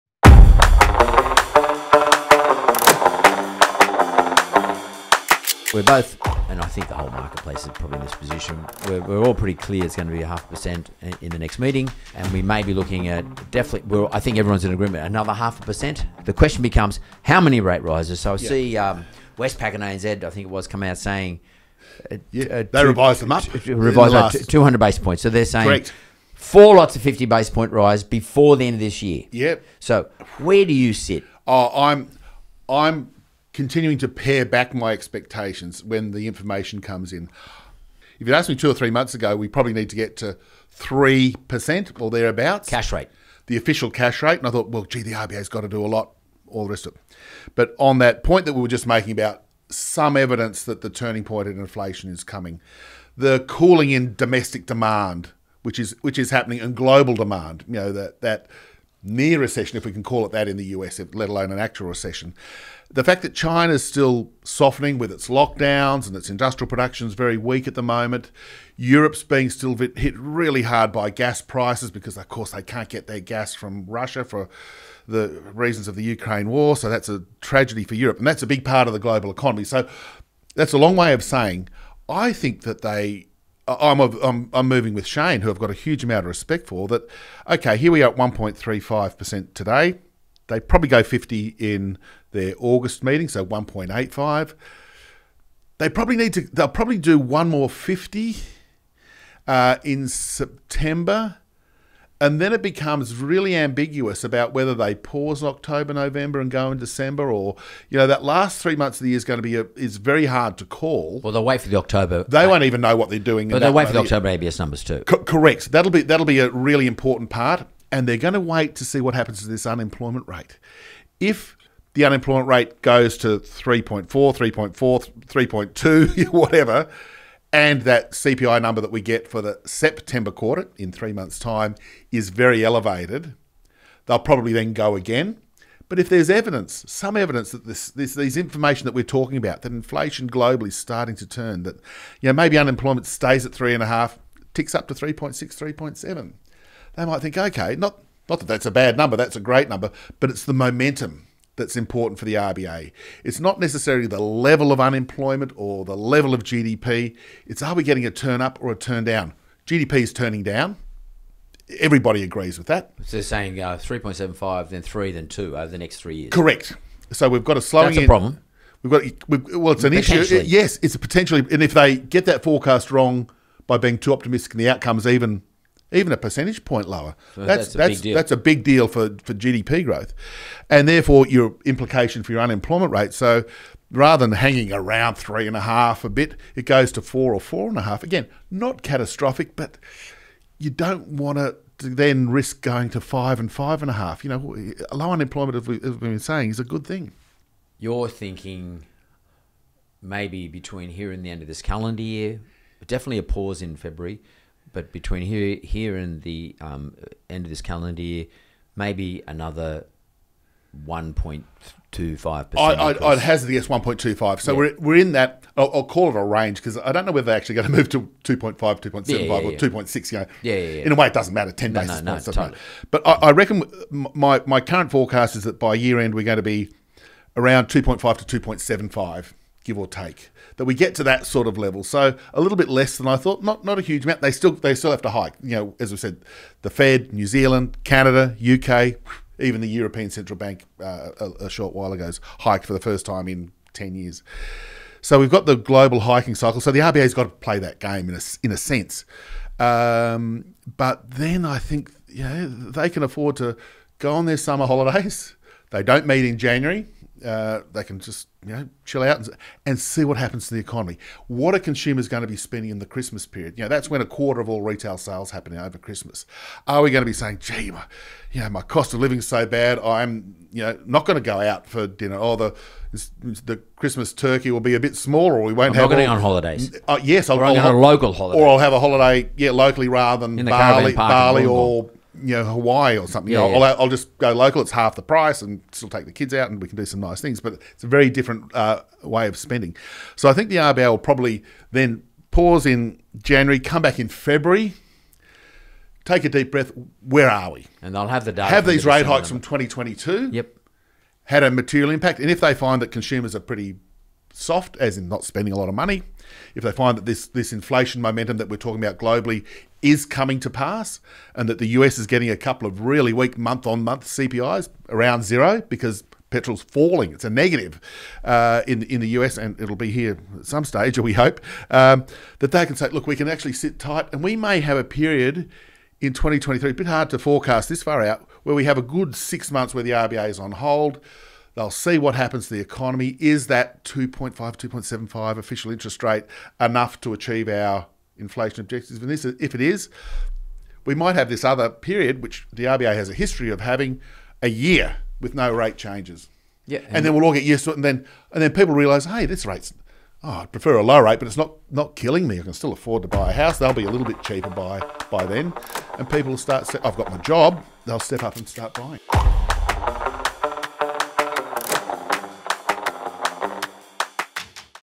Interest Rate Forecast: Beyond August - Mark Bouris & Stephen Koukoulas
Recorded 29 July 2022: Mark discusses the overall outlook for interest rates in August and beyond with Senior Economist Stephen Koukoulas.